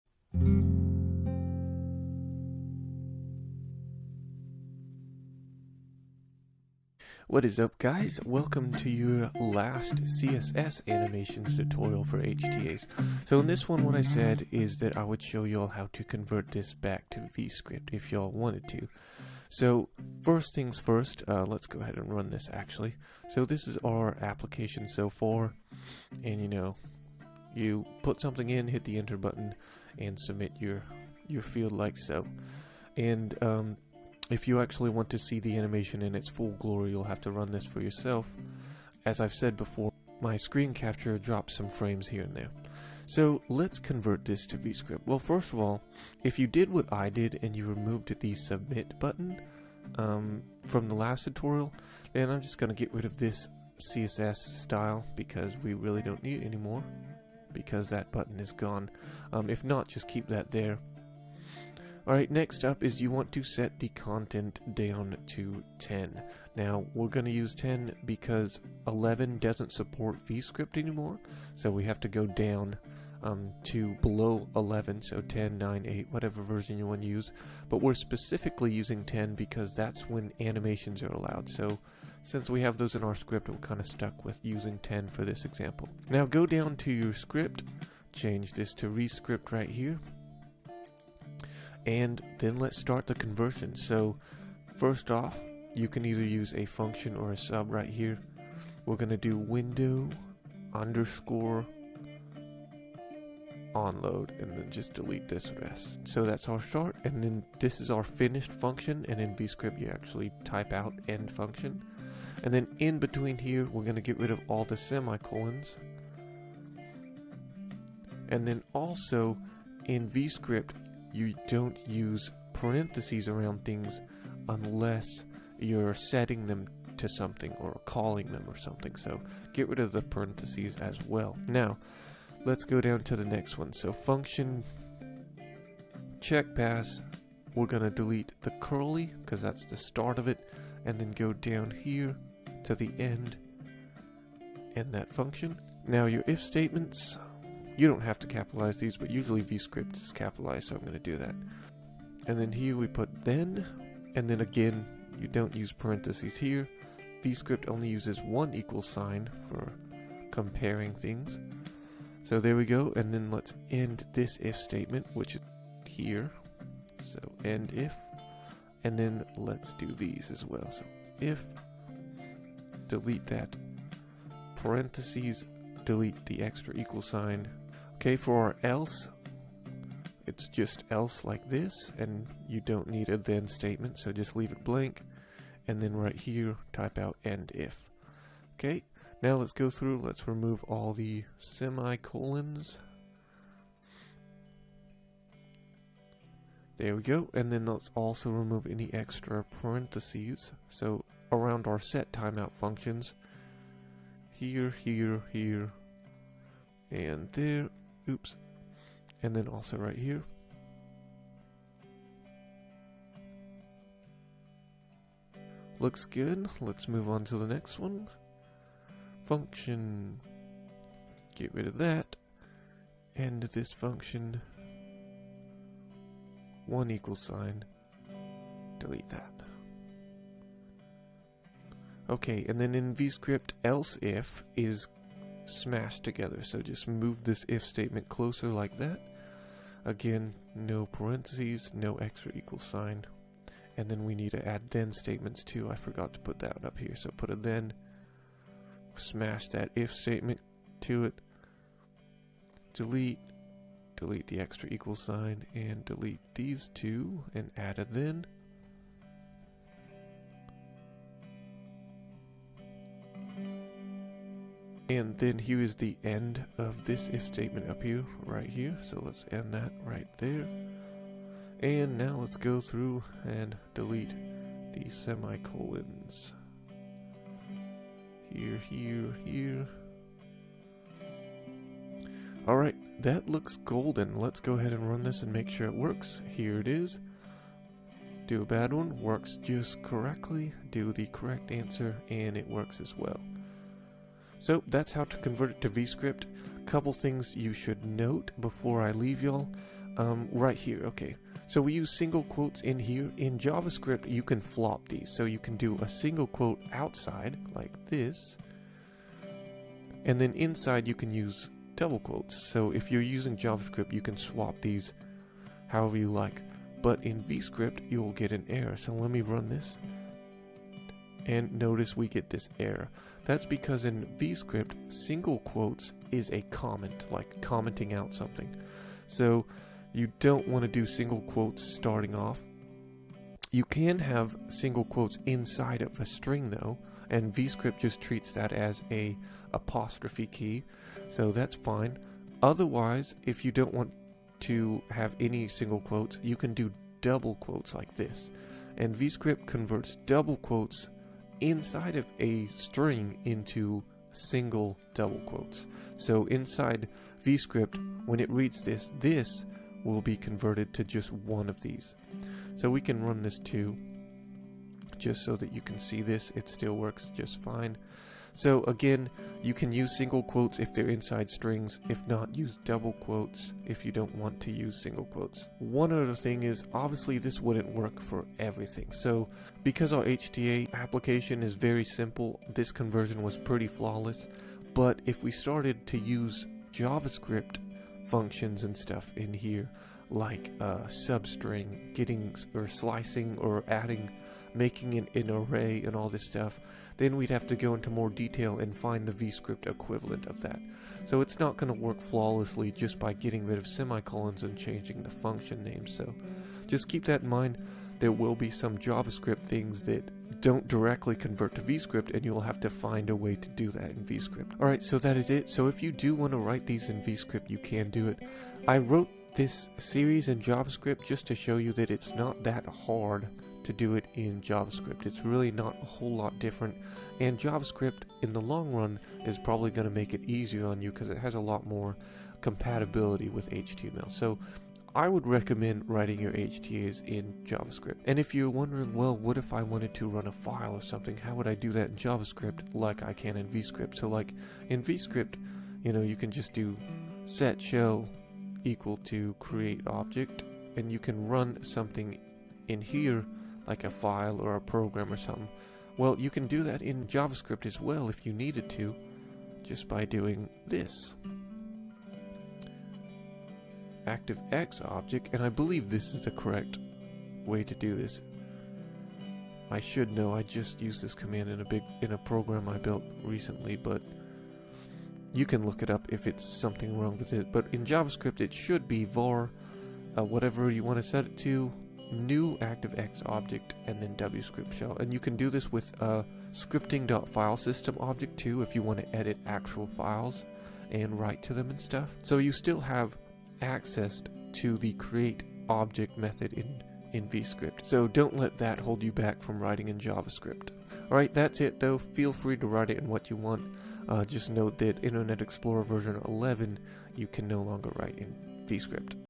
HTA Basics, Part 17 | Convert to vbscript (CSS Animations)
HTML Applications (.hta) Basic Tutorial:
This tutorial converts a javascript powered hta into a vbscript powered hta file.